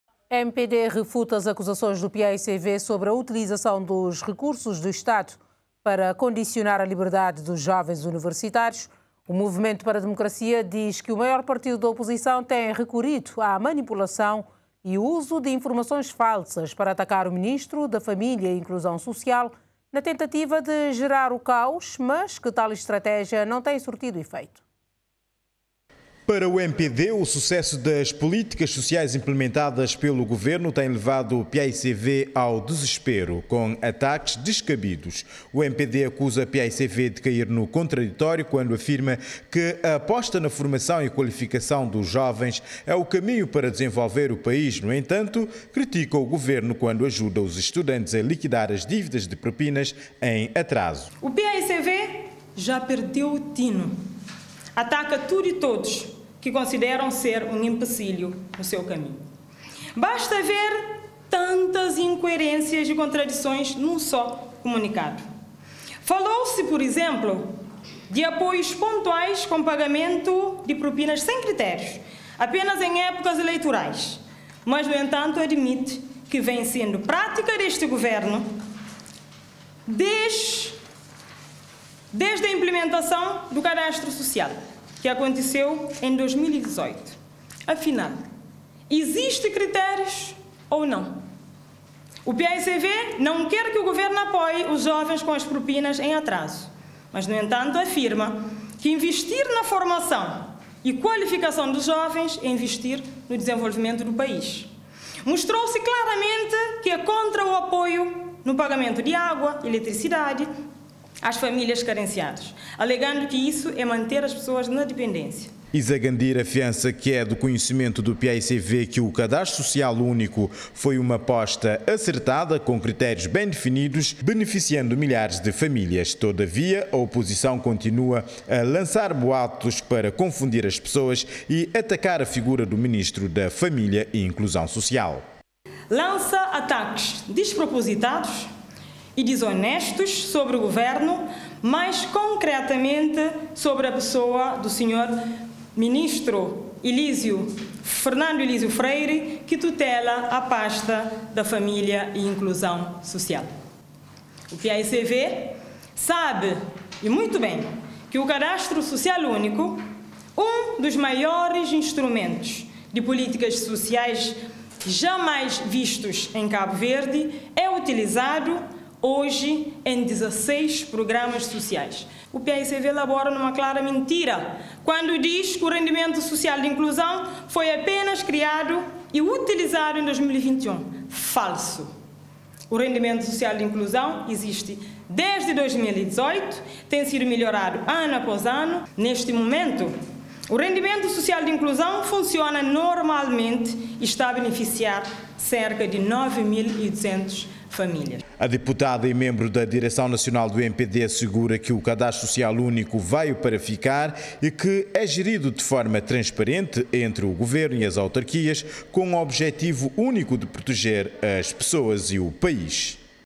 MpD refuta acusações do PAICV de utilização de recursos do Estado
MpD refuta acusações do PAICV de utilização de recursos do Estado para condicionar a liberdade de jovens universitários

RTC Online. Aqui, você está em Cabo Verde!